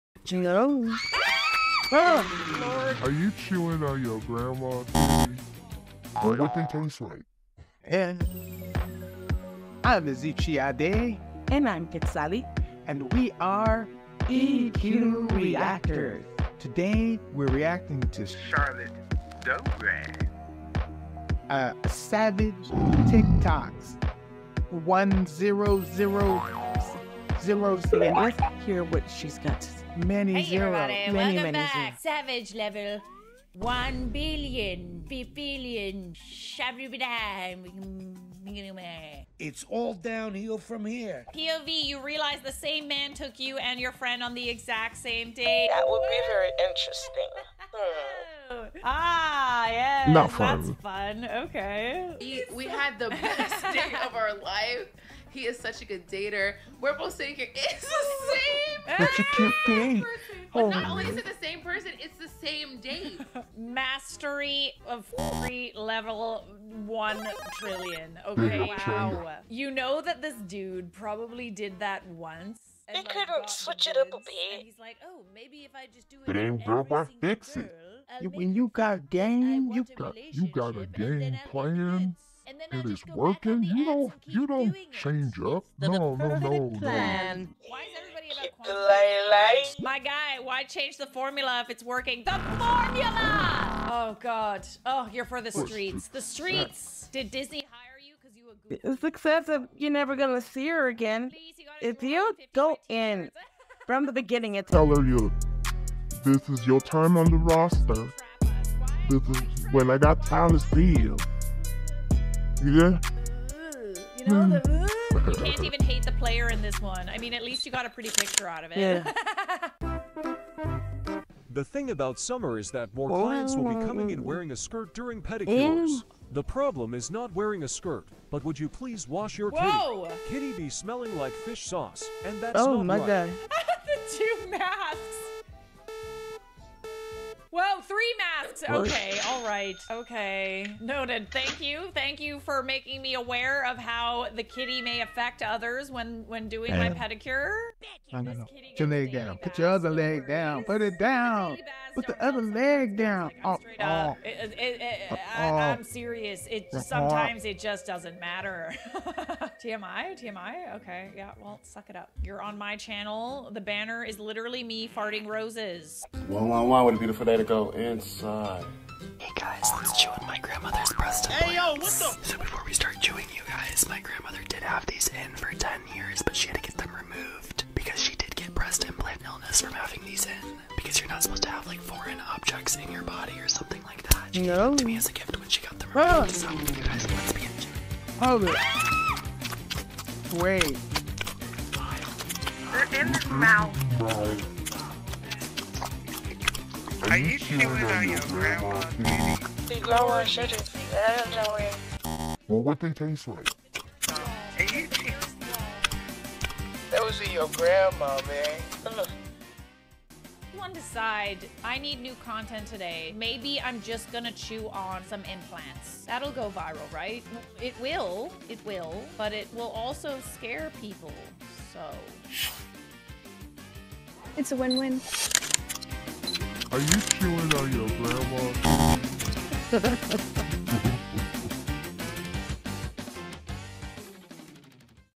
Epicly Savage Tiktoks Unveiled: Charlotte Dobre Vs. Ezichi & Quetzali!
EQ Reactors takes on Charlotte Dobre. Brace yourselves for uncontrollable laughter. In this censored for mature language video, from jaw-dropping moments to hilarious commentary, this video is a rollercoaster of emotions and laughter. Watch as eq reactors navigates through Charlotte's savage world with the perfect mix of shock, humor, and amusement. So grab your popcorn, prepare for belly laughs, and join eq reactors on this wild ride of a reaction video. And remember, when it comes to laughter, there are no limits – not even when it involves chewing on Grandma's breast implant! Get ready for the ultimate entertainment – it's time for some tiktok savage level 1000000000 reactions! 🤣👵💥